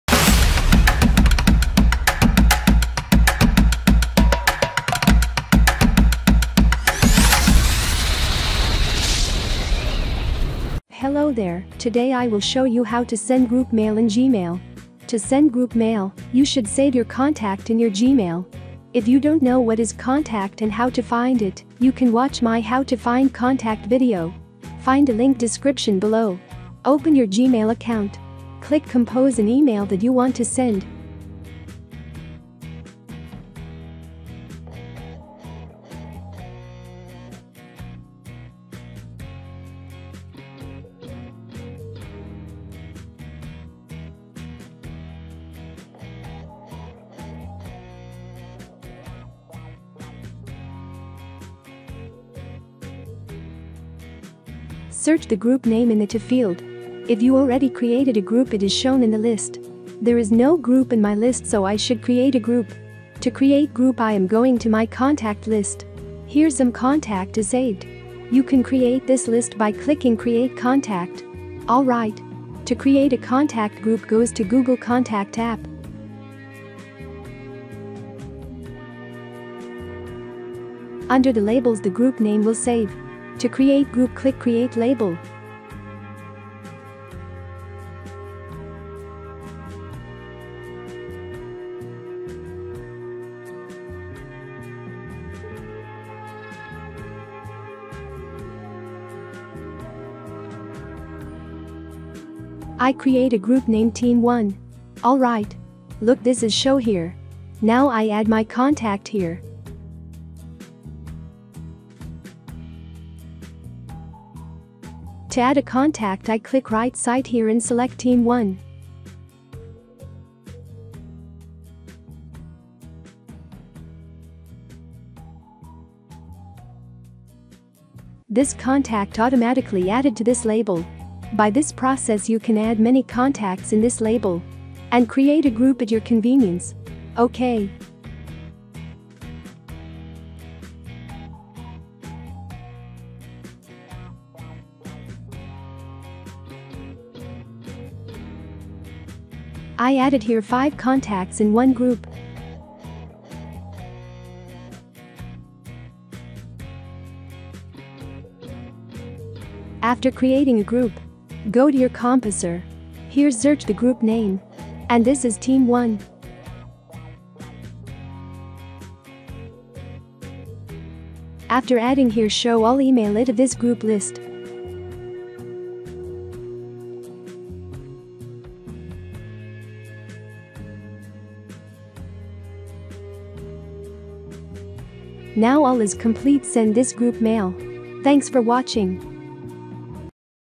How to Send a Group Email and Create Group in Gmail
Sending a group mail in Gmail is so easy. You can create a group by saving your Gmail contact. If you have many teams at work. This is very helpful for your time-saving. Mail does not have to be added one by one. Every mail you can save your time. Let's Watch this video.
Today I will show you how to send group mail in Gmail.
To send group mail, you should save your contact in your Gmail. If you don’t know what is contact and how to find it you can watch my “how to find contact” video. Find the link description below.
Open your Gmail account. Click compose an email that you want to send. Search the group name in the to field. If you already created a group it is shown in the list. There is no group in my list so I should create a group. To create group I am going to my contact list. Here some contact is saved. You can create this list by clicking create contact. All right.
To create a contact group goes to Google contact app. Under the labels the group name will save. To create group click create label. I create a group named team 1. All right.
Look this is show here. Now I add my contact here. To add a contact I click right site here and select team 1. This contact automatically added to this label. By this process you can add many contacts in this label. And create a group at your convenience.  Ok.
I added here five contacts in one group. After creating a group. Go to your composer. Here search the group name. And this is team One.
Now all is complete send this group mail.